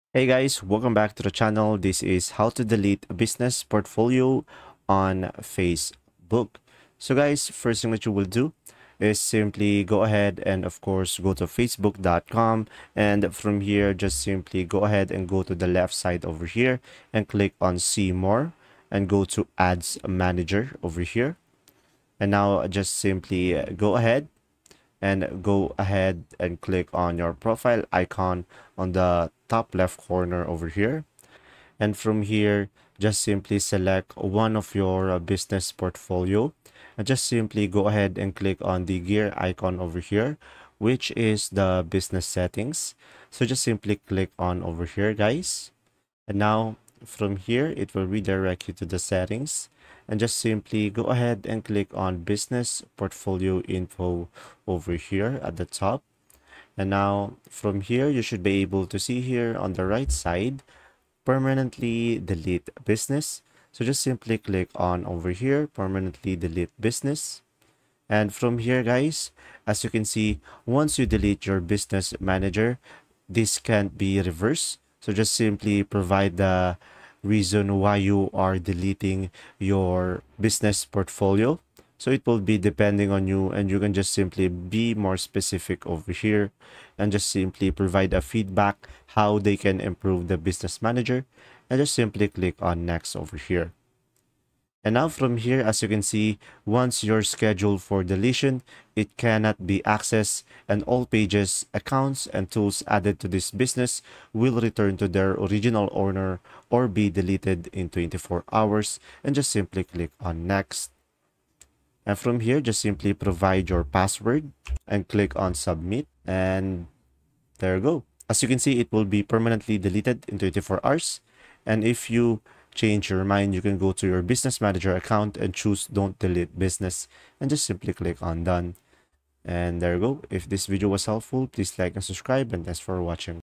How to delete business portfolio on Facebook (Easy) 2025
How to delete business portfolio on Facebook (Easy)

Learn how to easily delete your business portfolio on Facebook in this quick tutorial. If you're looking to remove your business page from Facebook, this video will show you how to do it step by step. Don't let your old business portfolio clutter up your Facebook page - watch this video to clean it up in no time!